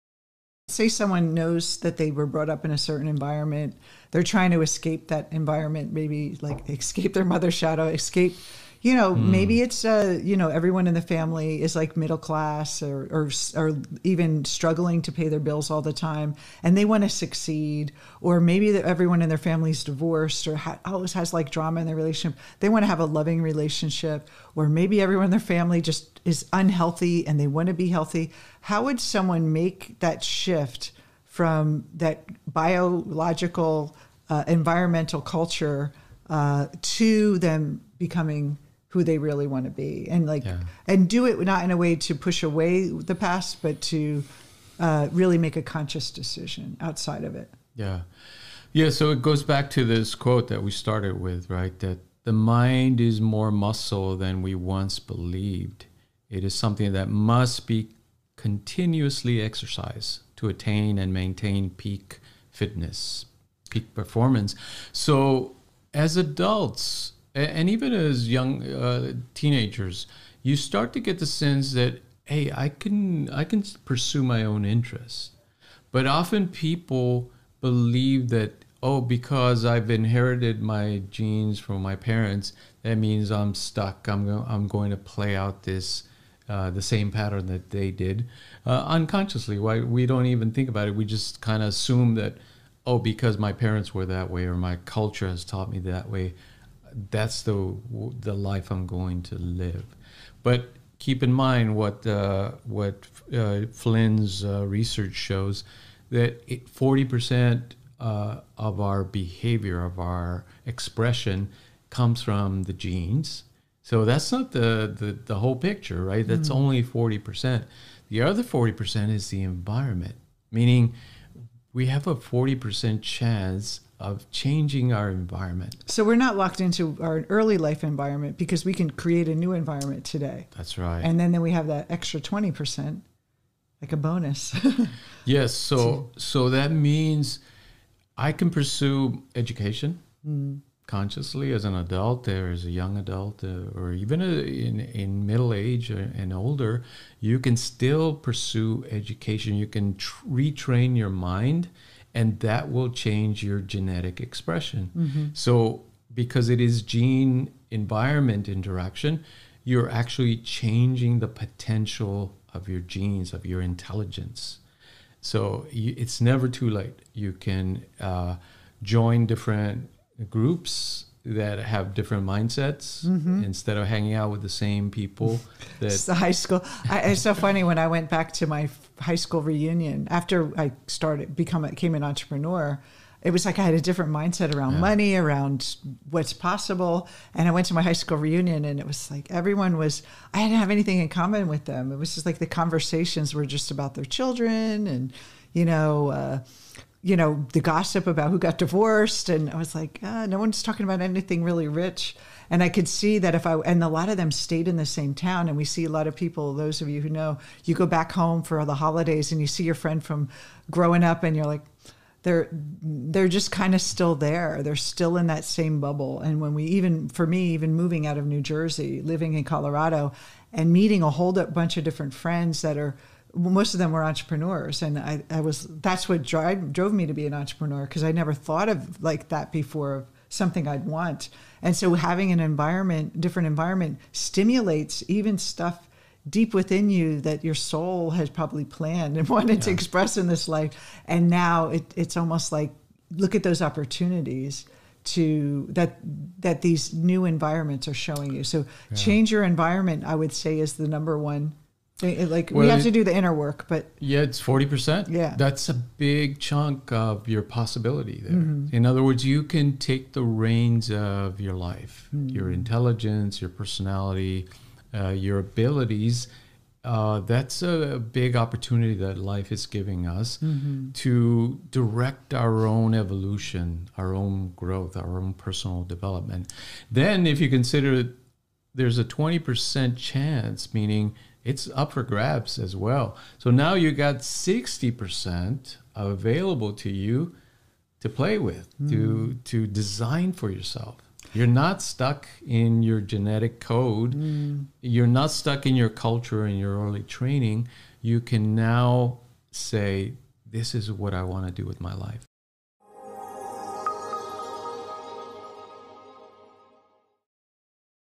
How to Break Free of Your Genetic Code | Jungian Life Coaching
“The mind is more muscle than we once believed. It is something that we must continuously exercise to attain and maintain peak fitness.” - James R. Flynn

Have you ever felt stuck from reaching your full potential because of the genes you inherited or the environment you grew up in? According to Flynn, our genes and our environment each comprise of forty percent of our potential. However, we are not locked into either percentage because we have the ability to change our environment and retrain our mind to change our genetic expression.

We can change our genetic expression by exercising our mind through learning and meditation. We can also change our environment by finding people that will push us towards achieving our goals.

Opportunities to Change Your Environment and Break Free of Your Genetic Code:

1. Meditate for ten minutes a day for one month.
2. Learn something new.
3. Find a community of like-minded people who will push you to reach your true potential

for courses and content.